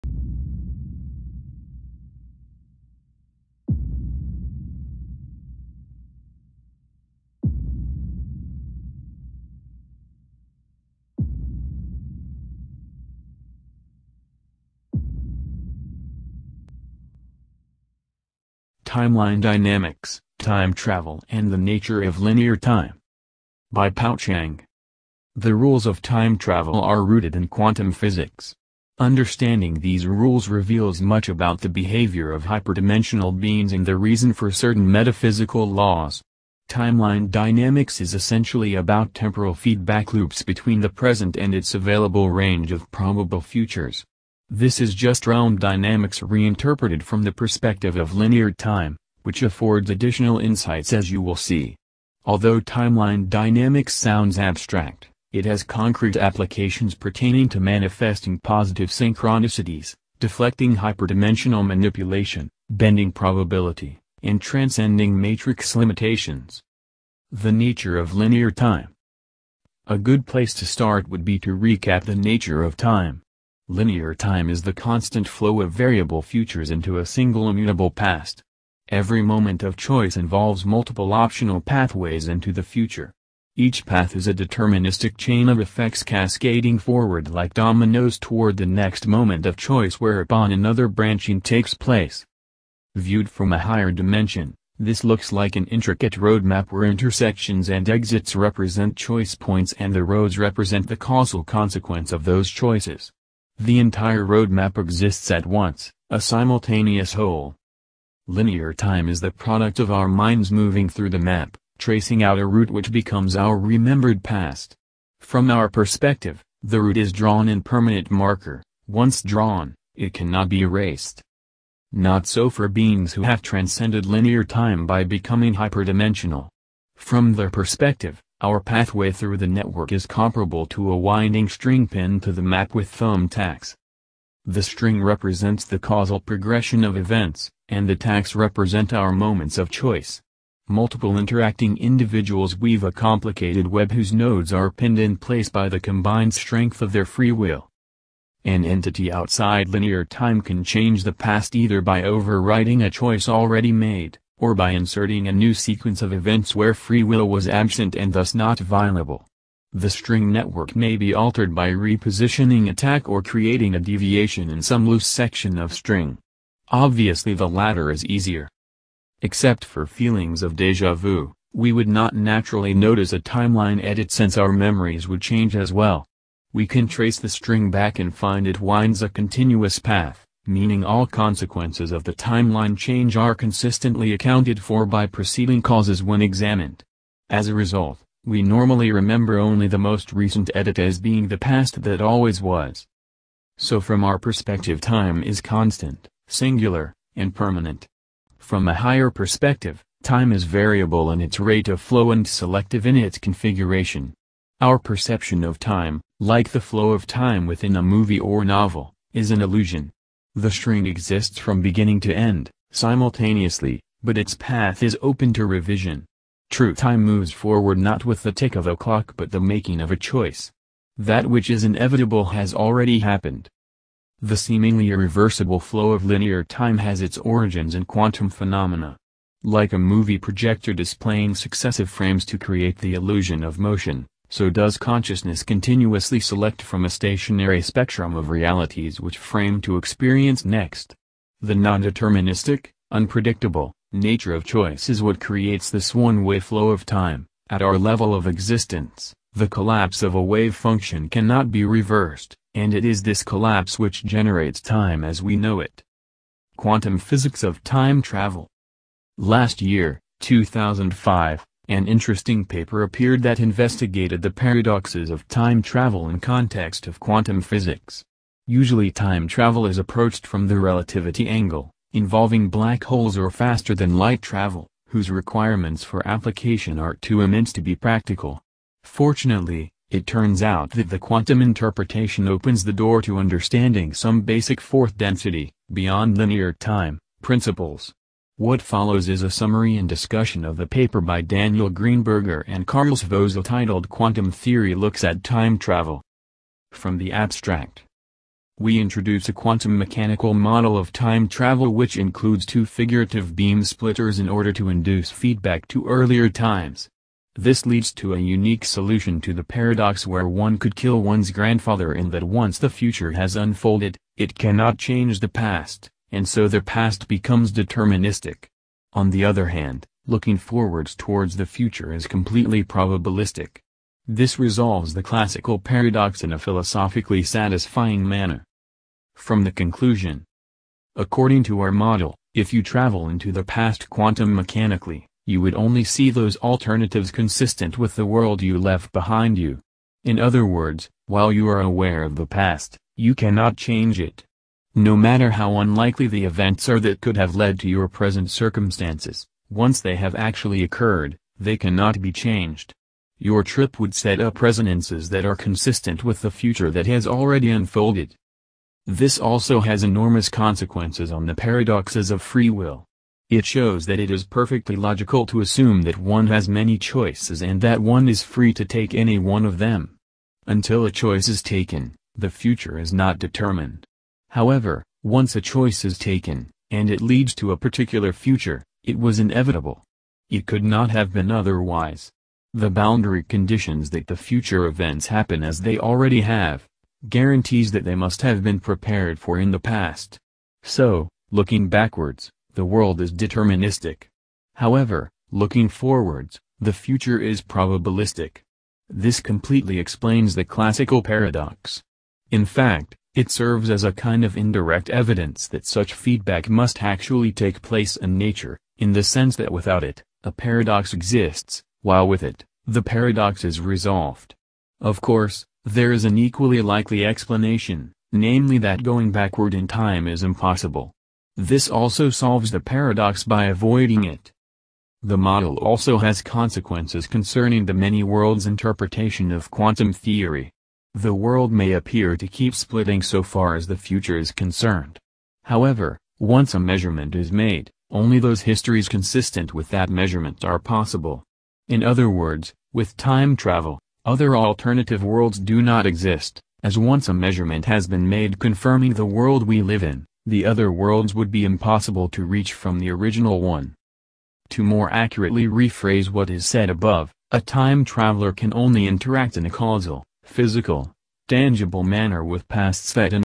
Quantum Jumping Time Travel and the Nature of Linear Time
The rules of time travel are rooted in quantum physics. Understanding these rules reveals much about the behavior of hyperdimensional beings and the reason for certain metaphysical laws. Timeline dynamics is essentially about temporal feedback loops between the present and its available range of probable futures. This is just realm dynamics reinterpreted from the perspective of linear time, which affords additional insights as you will see. Although timeline dynamics sounds abstract, it has concrete applications pertaining to manifesting positive synchronicities, deflecting hyperdimensional manipulation, bending probability, and transcending matrix limitations.

~ Buddha   : )